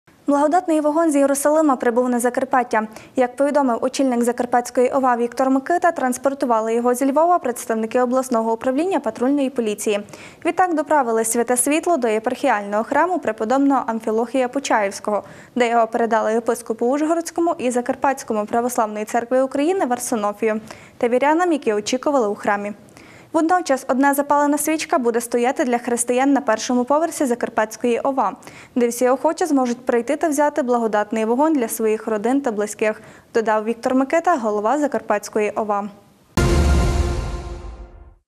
На Закарпаття прибув Благодатний вогонь. Символ свята
Благодатний вогонь з Єрусалима прибув на Закарпаття. Як повідомив очільник Закарпатської ОВА Віктор Микита, транспортували його зі Львова представники обласного Управління патрульної поліції. Відтак доправили Святе світло до Єпархіального храму преподобного Амфілохія Почаївського, де його передали єпископу Ужгородському і Закарпатському Православної Церкви України Варсонофію та вірянам, які очікували у храмі. «Водночас одна запалена свічка буде стояти для християн на першому поверсі Закарпатської ОВА, де всі охочі можуть прийти та взяти Благодатний вогонь для своїх родин та близьких», — додав Віктор Микита, голова Закарпатської ОВА.